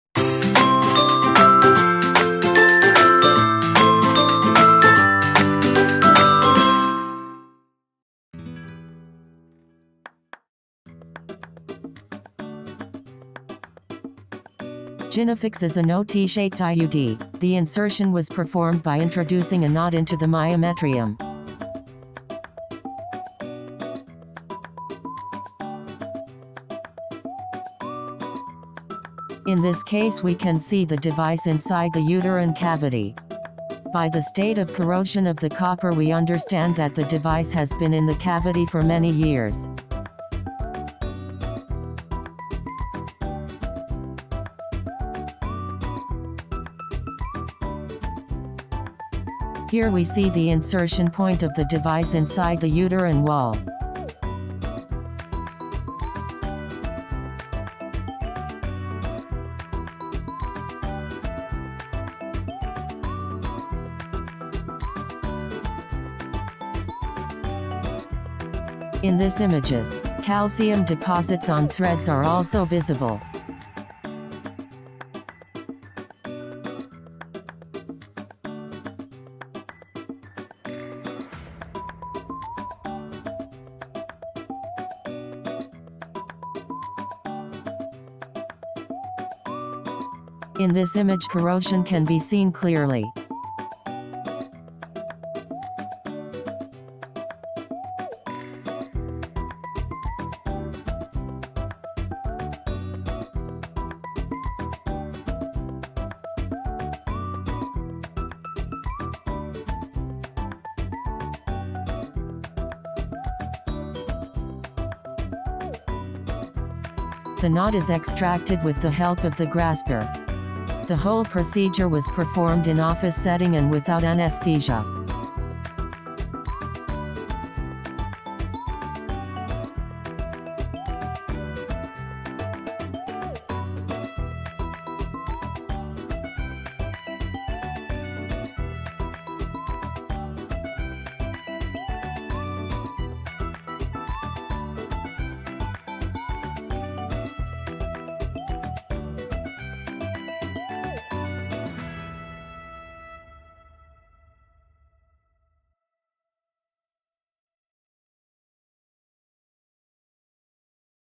Gynefix IUD assess by office hysteroscopy
IUD is a very popular contraceptive method, since the T-shaped devices appeared, little progress has been made. Recently an innovative spherical shaped device with copper pearls began to be used with many advantages over T shaped devices. Gynefix although in the market for more than 25 years, is not much in use in many countries, such as Spain or USA, where the T-shaped are far more popular. This is a rare case of hysteroscopic removal of this kind of device.